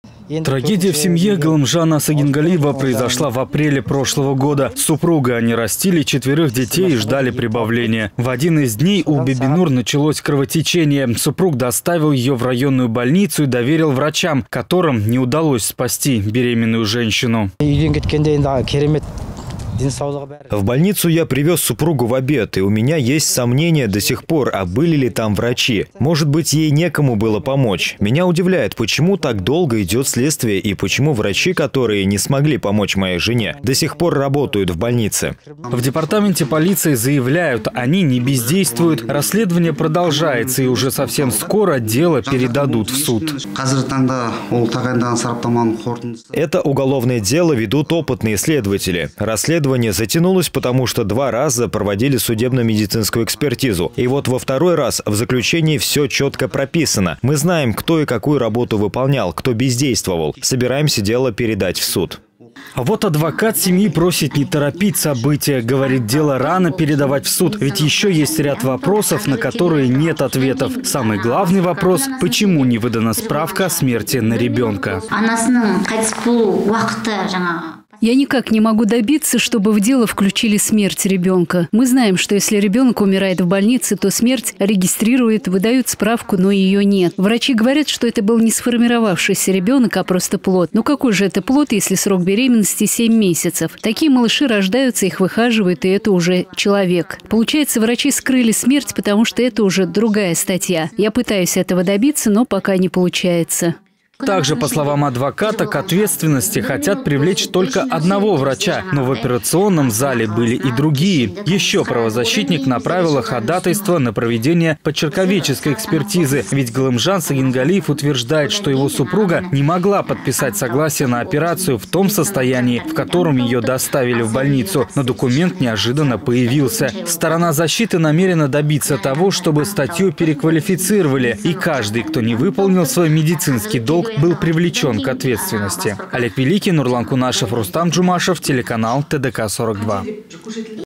Житель ЗКО винит врачей в смерти жены и ребёнка
Житель Жангалинского района уже больше года не может привлечь к ответственности виновных в смерти его супруги и не рожденного ребенка. Трагедия произошла в апреле прошлого года. Галымжан Сагингалиев и его супруга растили четверых детей и ждали прибавления. В один из дней у Бибинур началось кровотечение, супруг доставил ее в районную больницу и доверил врачам, которым не удалось спасти беременную женщину.